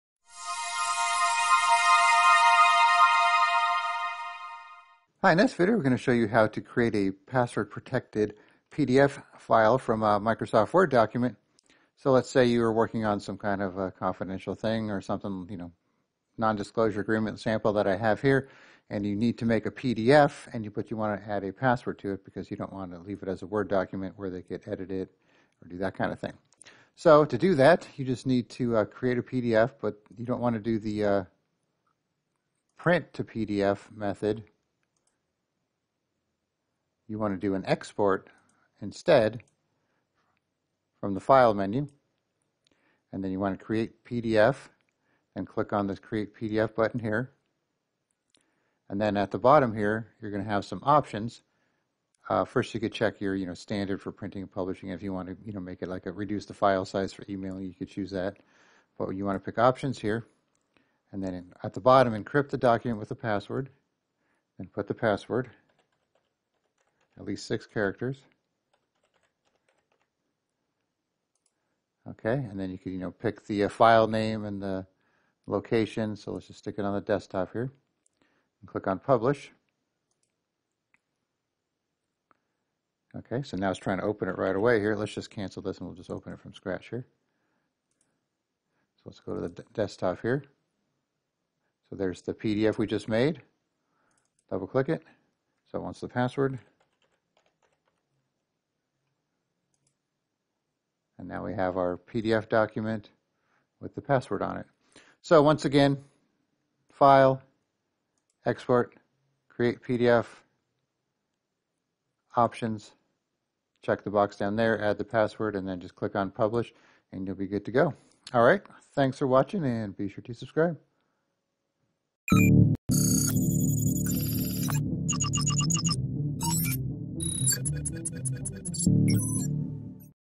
How to Create a Password Protected PDF Document in Microsoft Word or Other Office Apps
If you need to save a Word or other Office document or file as a PDF file, then it's easy to do by using the Microsoft Print to PDF function. But if you need to add a password to your PDF file then you will need to use the export option and then add a password before creating the PDF file itself. Then when someone tries to open your file, they will need to type in the password before being able to do so. This will work for Excel spreadsheets and other Office applications.

Check out our book (Office Made Easy) that is designed to take your Microsoft Office skills to the next level!